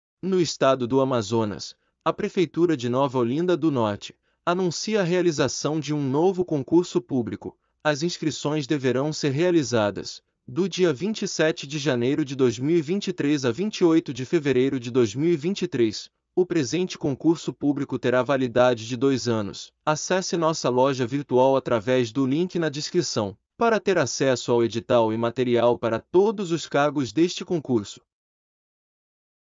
Apostila PREFEITURA DE NOVA OLINDA DO NORTE AM 2023 Professor Fundamental II Português
No estado do Amazonas, a Prefeitura de Nova Olinda do Norte, divulgou a abertura das inscrições de dois Concursos Públicos, destinados a contratação de profissionais de níveis fundamental incompleto e completo, médio, técnico e superior.

Os interessados devem se inscrever no período de 27 de janeiro de 2023 a 28 de fevereiro de 2023, até as 23h59, exclusivamente pelo site do Instituto Merkabah, sendo cobrada taxa nos valores de R$ 60,00 a R$ 100,00.

➔ Cadastre-se para receber atualizações de todos os concursos 2023. Concurso  PREFEITURA DE NOVA OLINDA DO NORTE AM 2023👇